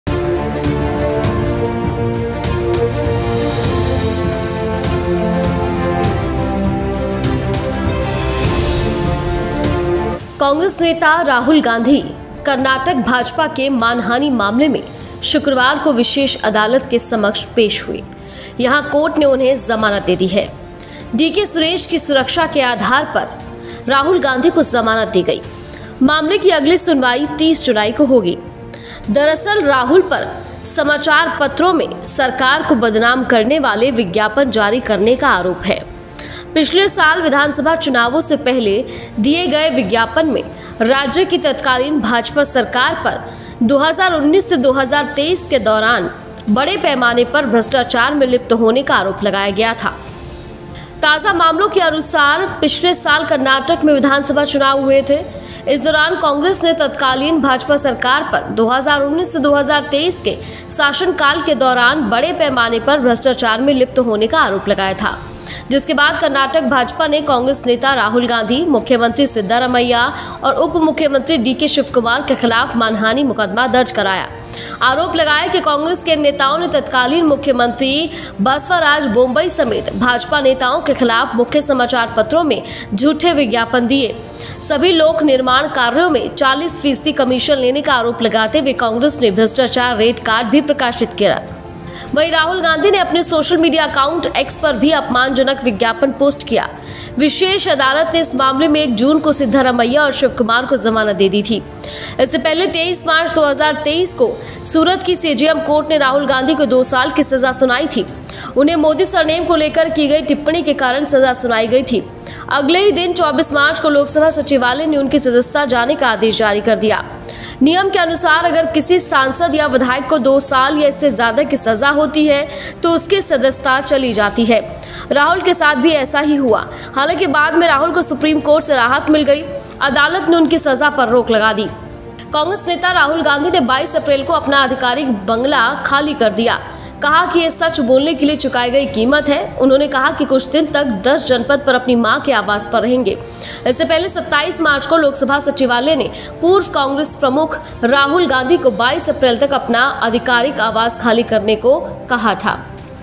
राहुल गांधी को मिली जमानत | RAHUL GANDHI | KARNATAK | CONGRESS
Congress leader Rahul Gandhi appeared before a special court on Friday in the Karnataka BJP defamation case. Here the court granted him bail. Rahul Gandhi has been granted bail on the basis of DK Suresh's security. The next hearing of the case will be on July 30.